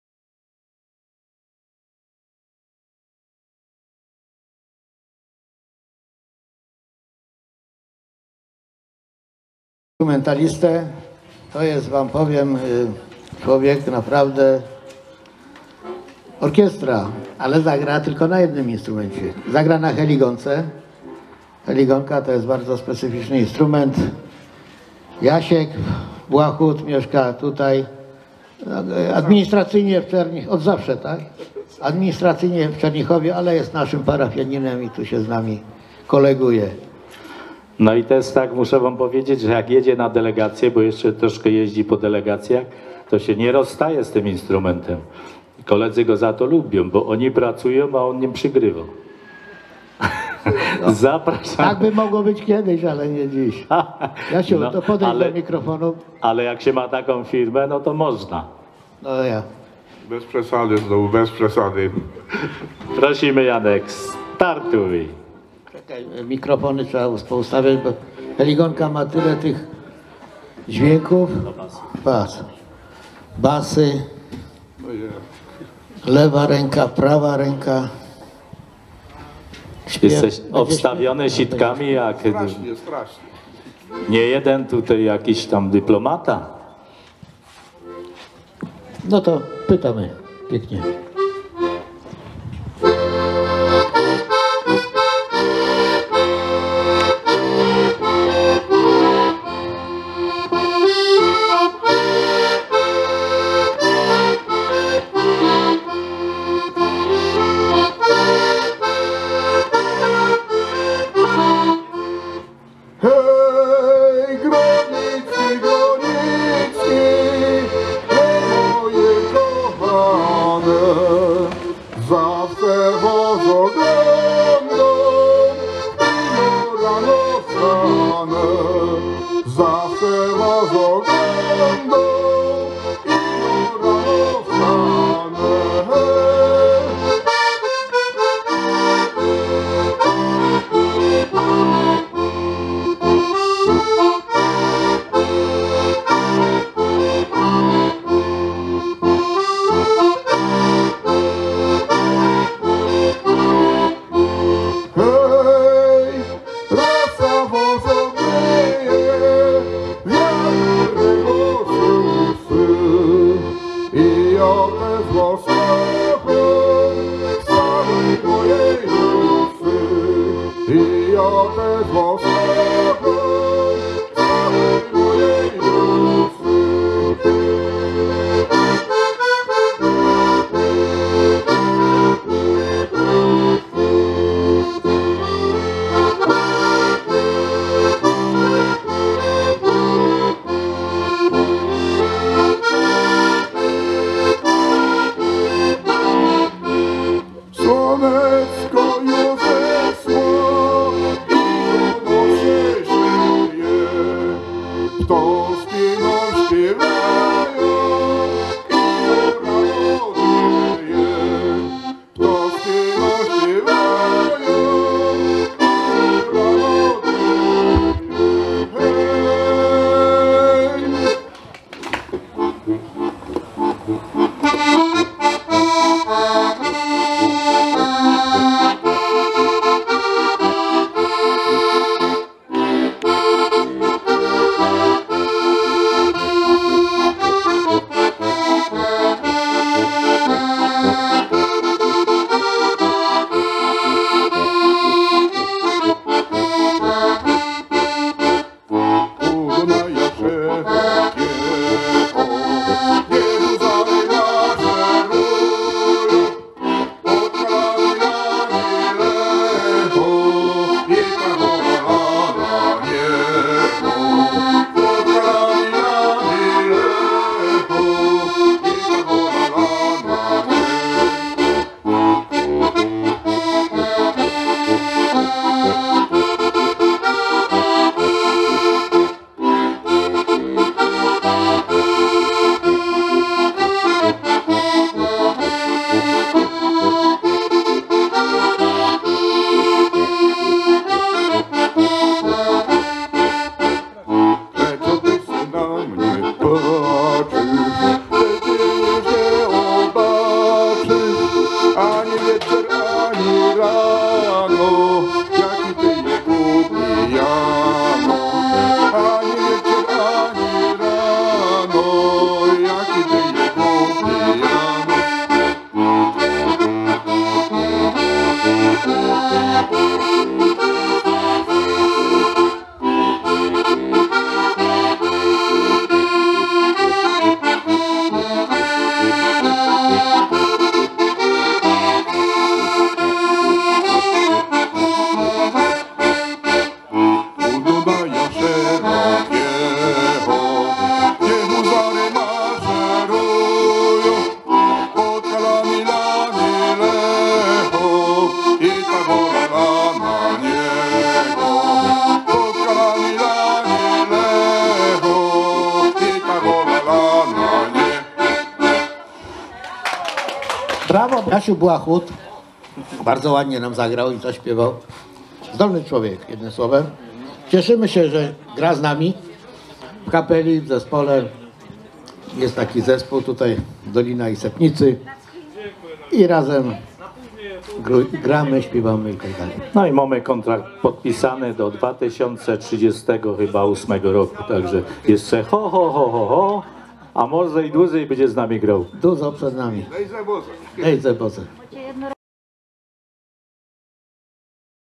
STANEK49 JAN BŁACHUT z CZERNICHOWA- "PRZĘGLĄDACKA 2021 MIEDZYBRODZIE ŻYWIECKIE 07.08.2021
IMPREZA ZOSTAŁA WPISANA DO "ROK GÓRALI w WOJEWÓDZTWIE ŚLASKIM w 2021"